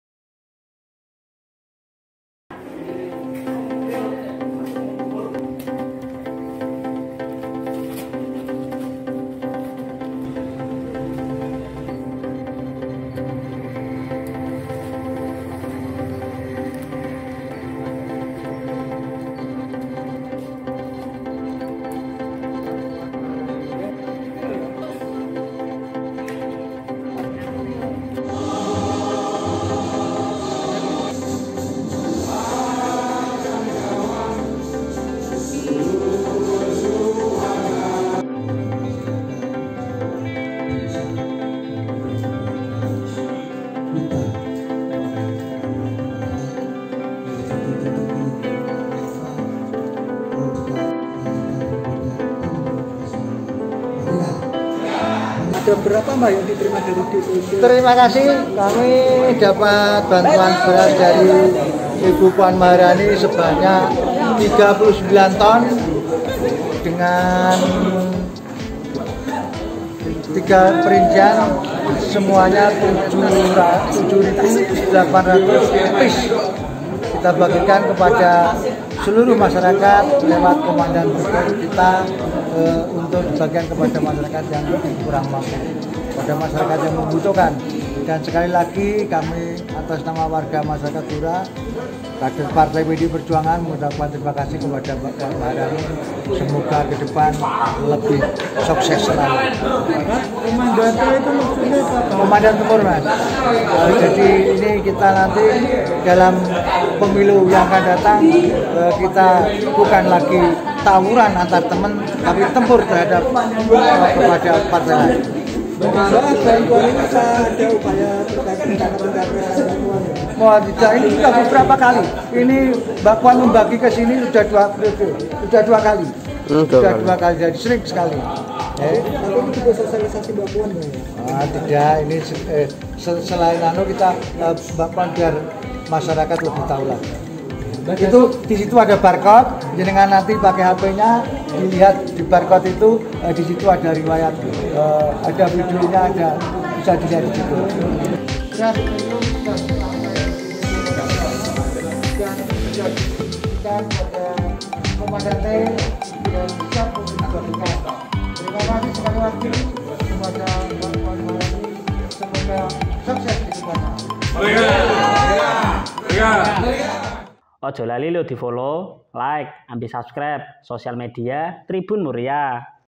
Jelang Lebaran, Puan Maharani Distribusikan 7 800 Paket Beras Lebaran Bagi Masyarakat Blora
Menjelang lebaran atau Hari Raya Idul Fitri 2022, Puan Maharani, mendistribusikan 39 ton atau 7.900 paket beras kepada warga di Kantor DPC PDI Perjuangan Blora.

Beras ini akan dibagikan kepada seluruh masyarakat lewat komandan tempur kepada warga yang kurang mampu.

Dasum membantah bantuan ini terkait rencana Pencapresan Puan Maharani dalam Pemilu 2024 mendatang.

Pihaknya mengakui bantuan ini lebih mendekatkan Puan Maharani ke masyarakat.